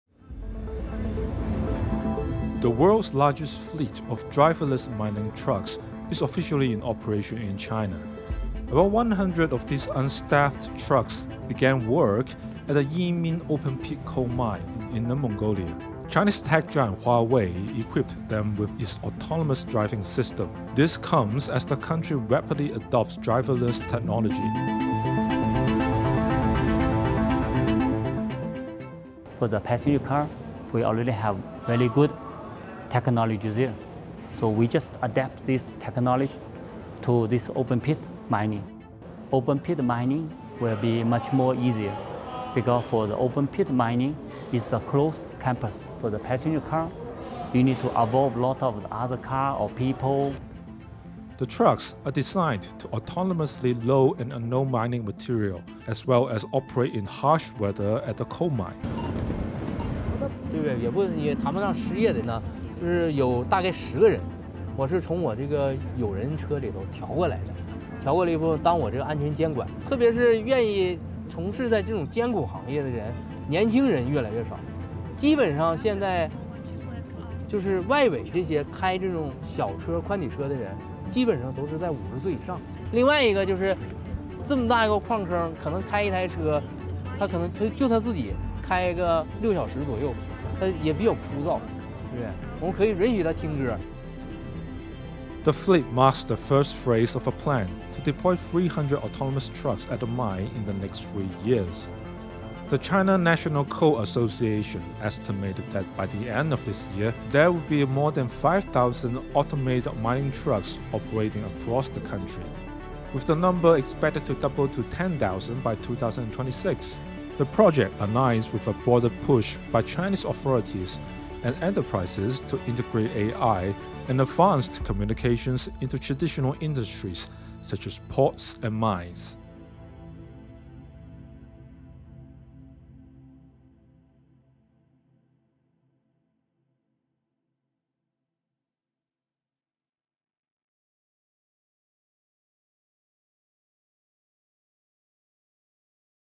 Driverless mining trucks begin operating in China
Chinese state-backed energy giant Huaneng Group has launched the world’s largest fleet of driverless electric mining trucks in China’s province of Inner Mongolia. Some 100 vehicles are equipped with autonomous driving systems developed by Huawei Technologies. In recent years, Chinese companies and authorities have been pushing to develop and implement such technology.